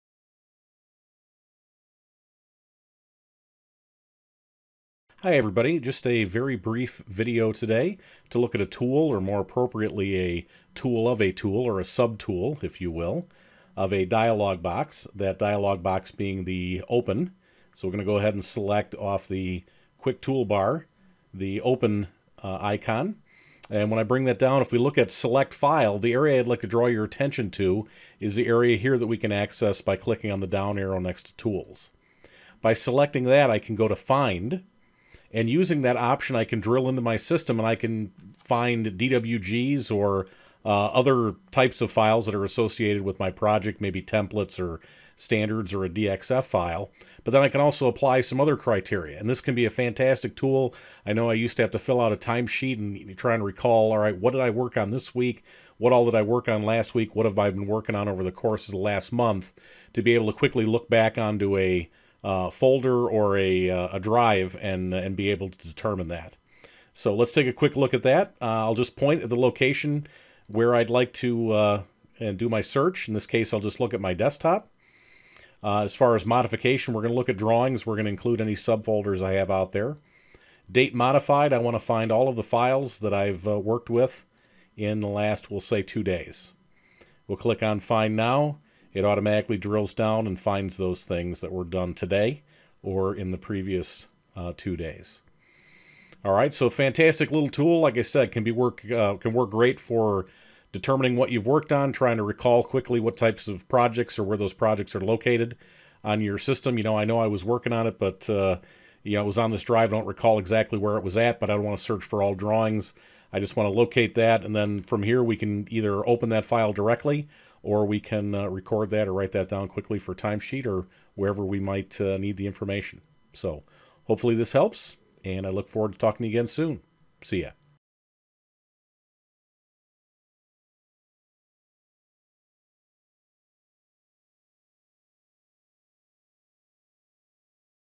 How to search for DWGs using the Find Files option of the AutoCAD OPEN Dialog box.
In this (2 min) video, I show how to use a little known option of AutoCAD's OPEN Dialog box to find files on your system.  The context of this video is to find files that were created or modified over the past several days.  (To help facilitate the completion of a timesheet)  This tool can be used in any AutoCAD based application.  (Civil 3D, AutoCAD Architecture, Map 3D, Plant 3D...)